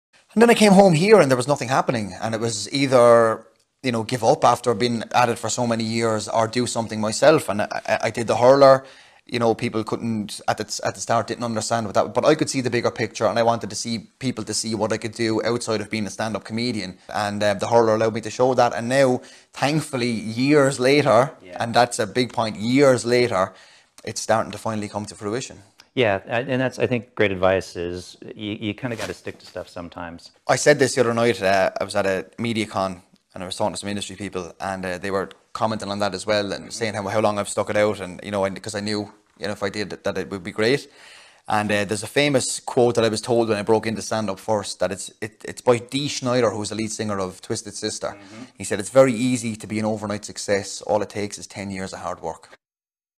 Tony Kelly on Not Giving Up
6 years ago we spoke with Tony Kelly about his web series, “The Hurler”. He gave us some advice about sticking it out, year after year, to follow your passion. Proving that to be true “The Hurler” is now a feature film showing in theaters this month.

to find out where you can watch and support independent filmmakers.